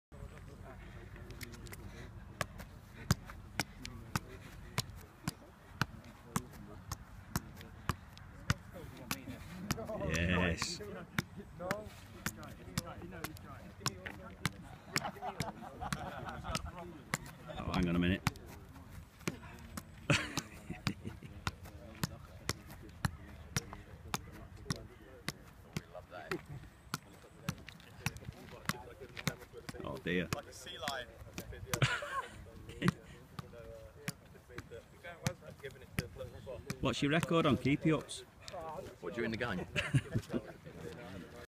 Birmingham City's Wes McDonald shows off his skills
Wes McDonald shows his freestyling skills after a session at PSV Eindhoven during Birmingham City U18's tour of The Netherlands.